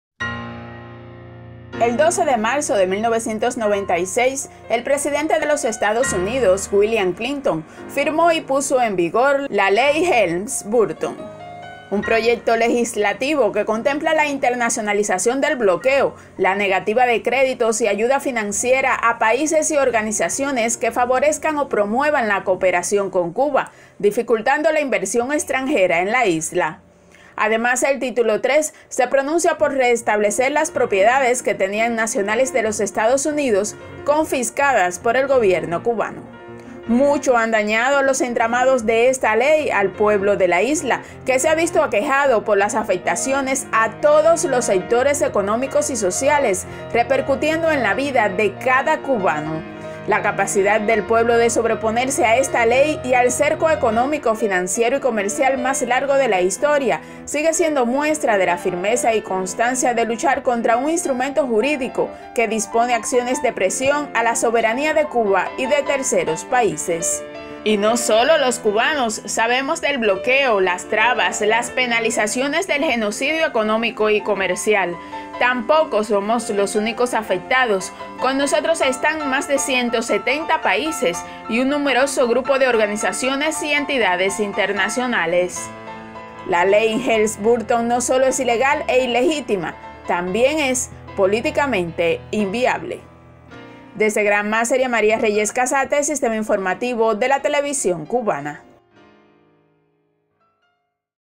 Ley Helms Burton viola flagrantemente las leyes y los derechos humanos del pueblo cubano
La Ley Helms-Burton viola flagrantemente las leyes y los derechos humanos del pueblo cubano, la Constitución de los Estados Unidos y varias normas jurídicas de ese país. Desde su promulgación hasta la fecha ha sido instrumento político para medidas que afectan el desarrollo económico y social del pueblo cubano